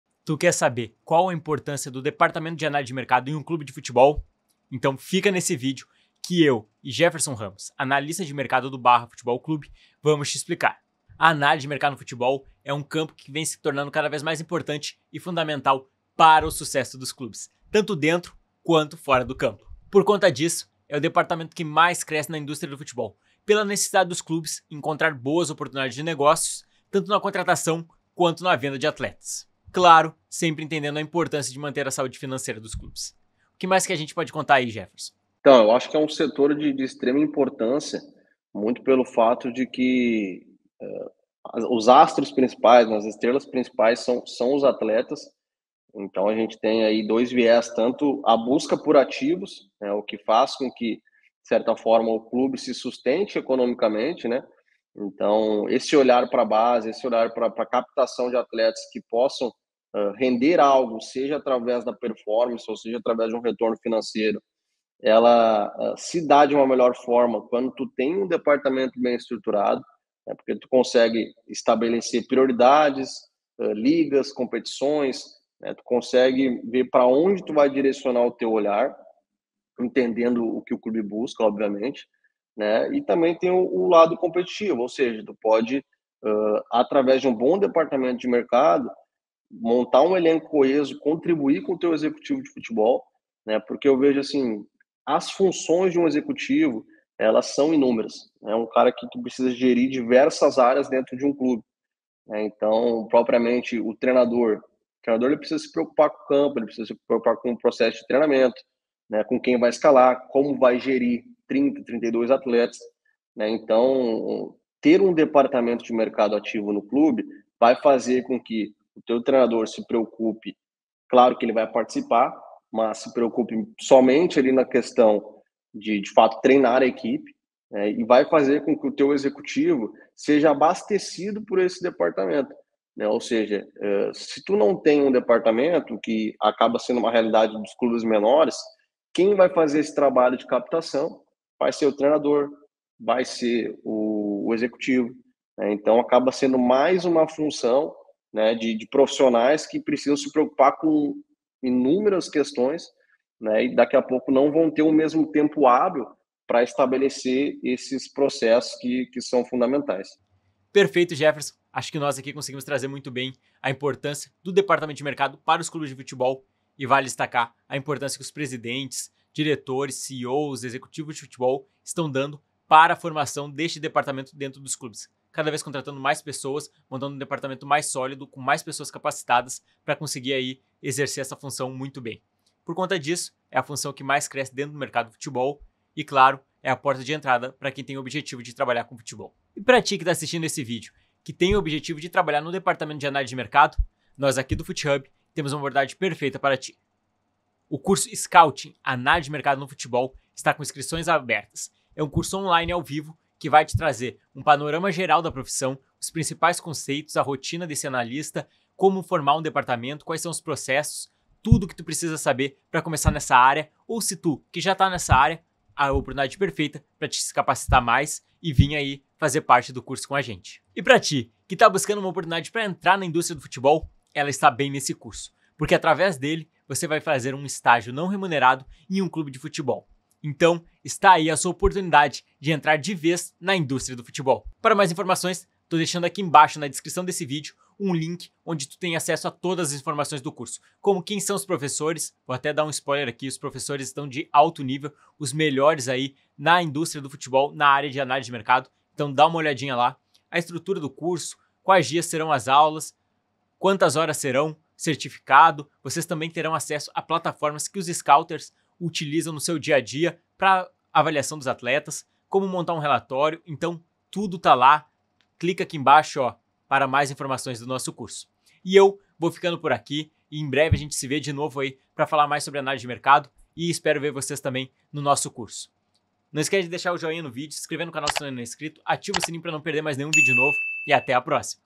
QUAL A IMPORTÂNCIA DA ANÁLISE DE MERCADO NO FUTEBOL? FH EXPLICA #1
No primeiro episódio do FootHub Explica trazemos como tema a "Importância da Análise de Mercado no Futebol", e contamos com a participação de Jeferson Ramos, analista de mercado do Barra FC, para explicar um pouco mais sobre esse departamento que é o mais cresce na Indústria do Futebol.

Link para mais informações do curso Scouting - Análise de Mercado no Futebol